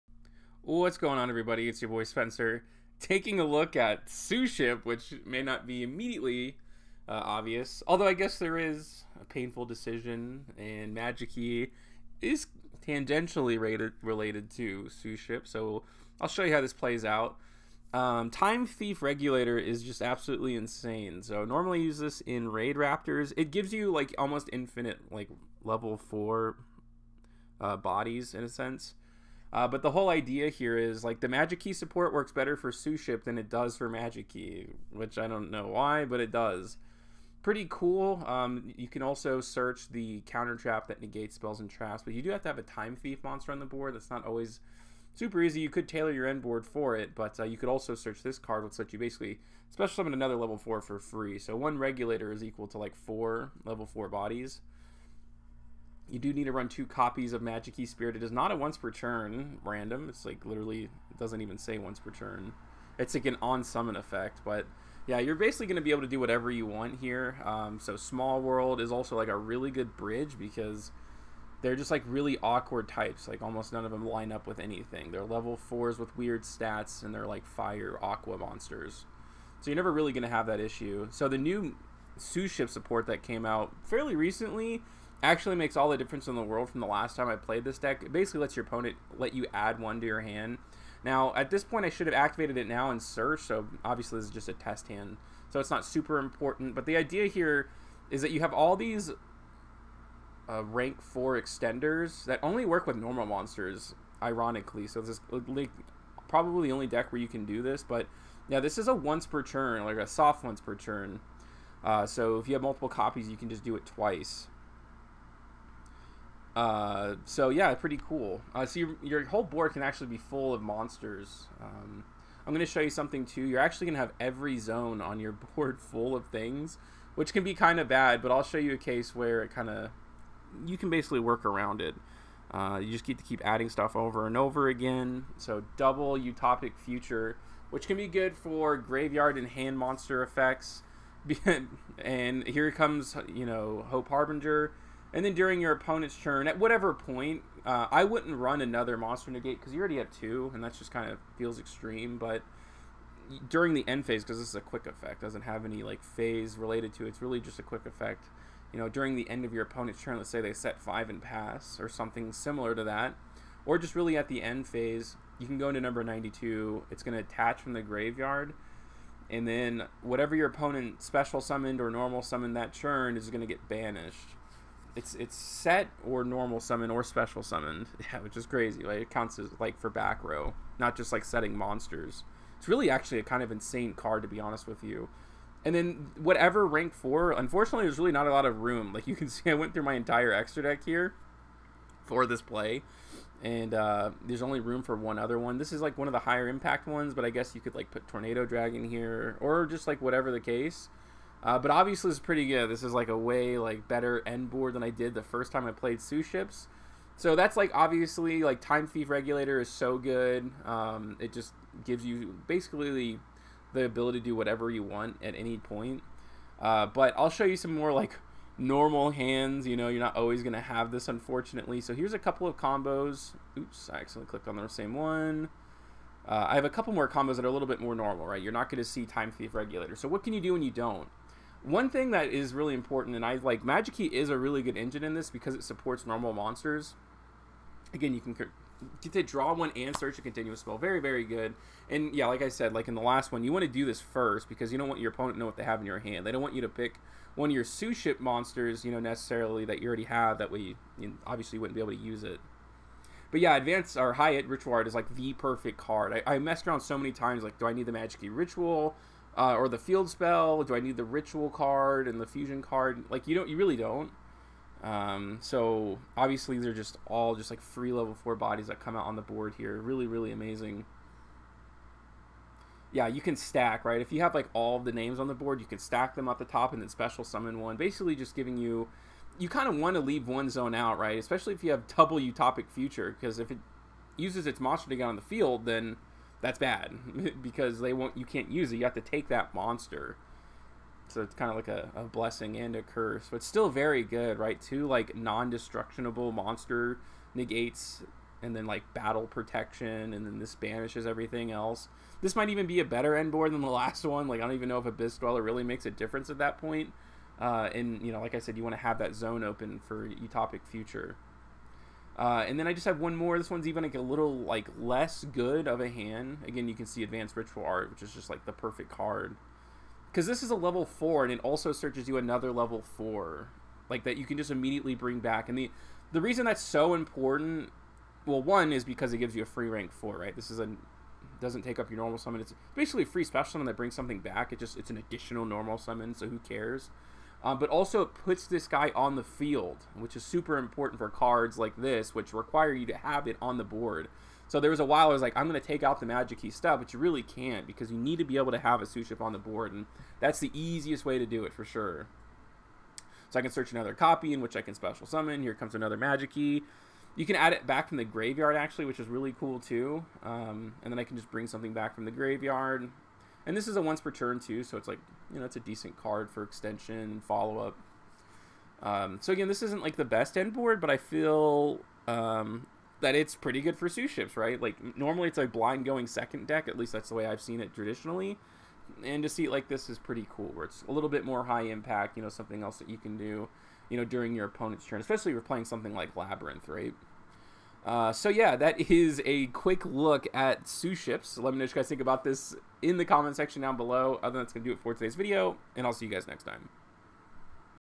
CRAZY Suship Combos You Need to See! | Yu-Gi-Oh! 2023
I love rank 4 strategies because all of them are so different even though they centralize around the same concepts.  Suship have the insane ability to create two F0: Utopic Future Dracos while still going full combo.  The newish continuous spell Gunkan Suship Catch of the Day is the icing on the cake to ensure that this deck is going end on some really crazy end boards.  Who doesn't want to see boss monsters in every single one of your available monster zones?